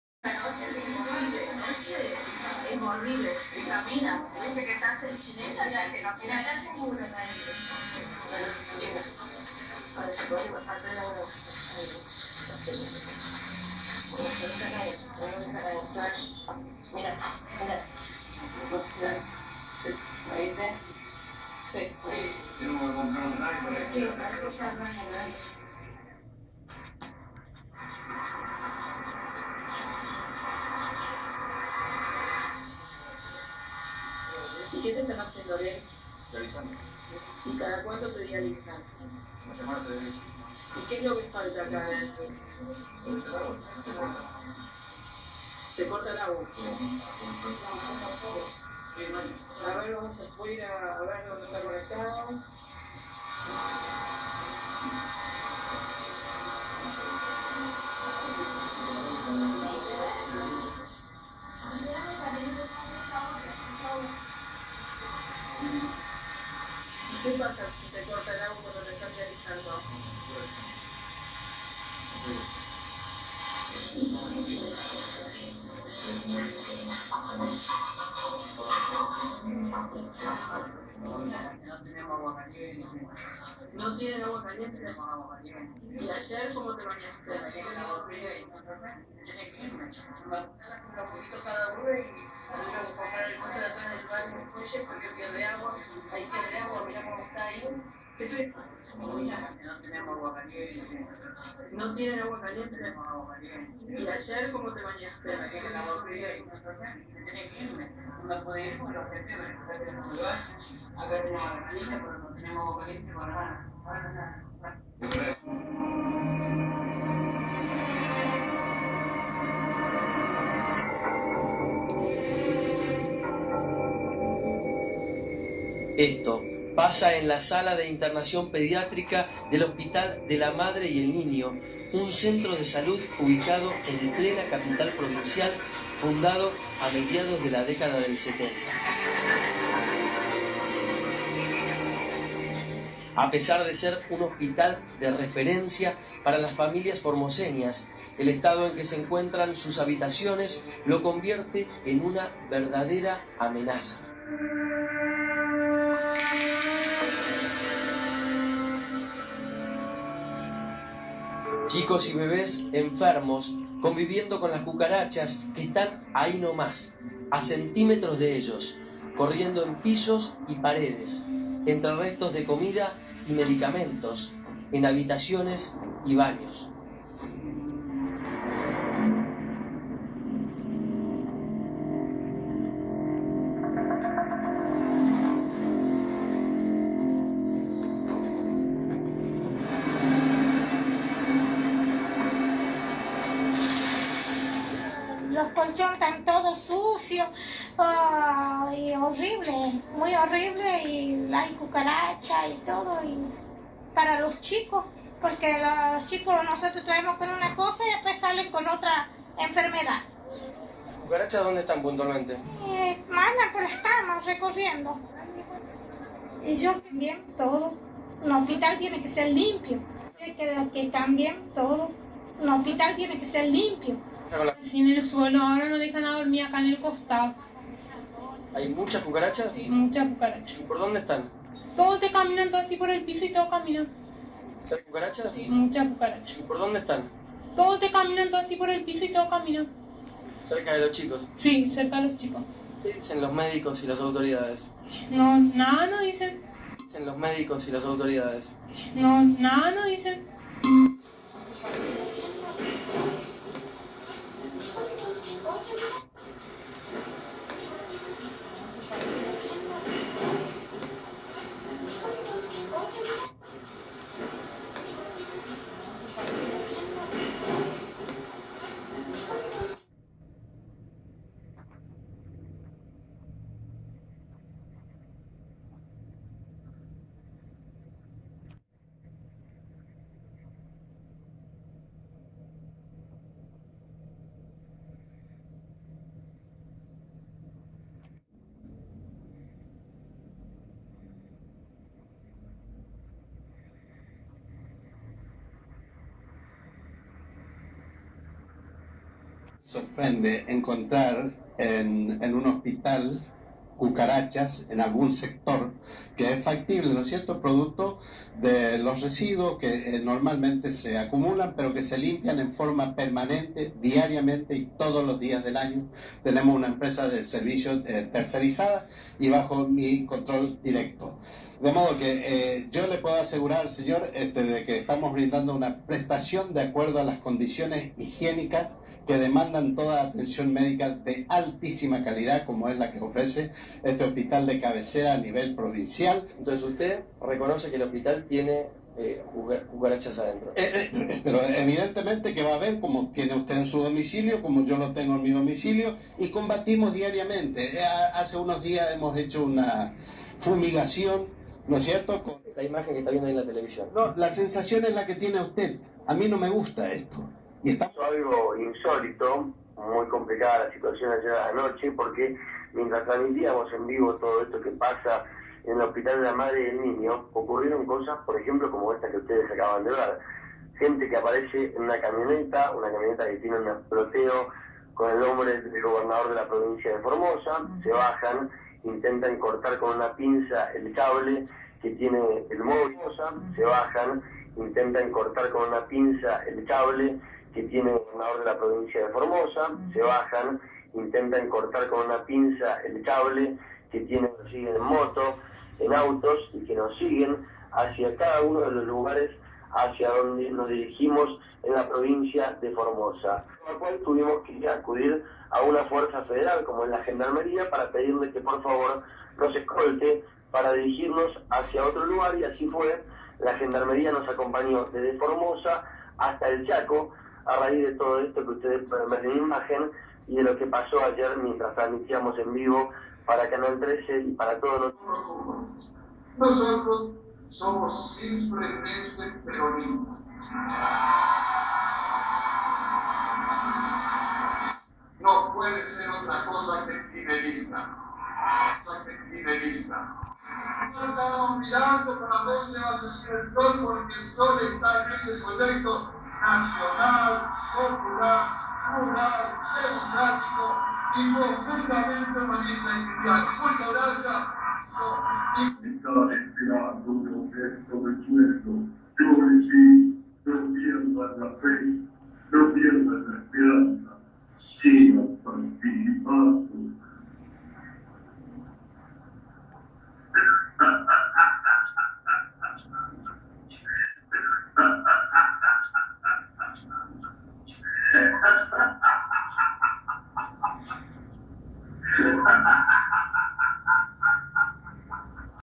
HOSPITALES GILDISTAS-K
LA PRESI E INSFRAN SE JACTAN DE SER HUMANISTAS Y PROFUNDAMENTE CRISTIANOS PERO EL "MODELO" ESTA LLENO DE MAGNATES, EMPLEADOS PUBLICOS CON RANGO FORRADOS PARA SIEMPRE!INSFRAN SE RASGA LAS VESTIDURAS ORGULLOSO DE SU MODELO "POPULAR" IMAGINARIO, PERO ESTE NO EXISTE,Y NO PARECERIA , SEGUN SE VE DESDE HACE AÑOS, HUMANIZARSE O CRISTIANIZARSE CON SU PROJIMO EN LOS HOSPITALES PÚBLICOS EN LOS QUE A PESAR DEL ESFUERZO DE MUCHOS TRABAJADORES DE LA SALUD QUE SE COMEN EL GARRON POR CULPA DEL REGIMEN, MUY POCO PUEDEN HACER ESNFERMERAS O MEDICOS SIN INSUMOS , ELEMENTOS E HIGIENE.